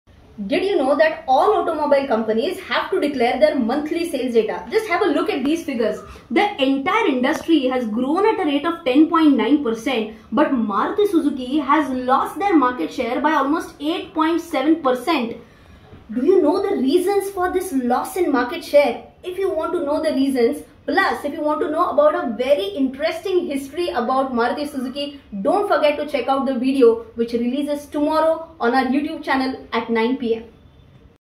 Maruti Suzuki Fundamental Analysis | CA Rachana Ranade
In tomorrow's video, you'll be learning about the reasons for the fall in Maruti Suzuki's share.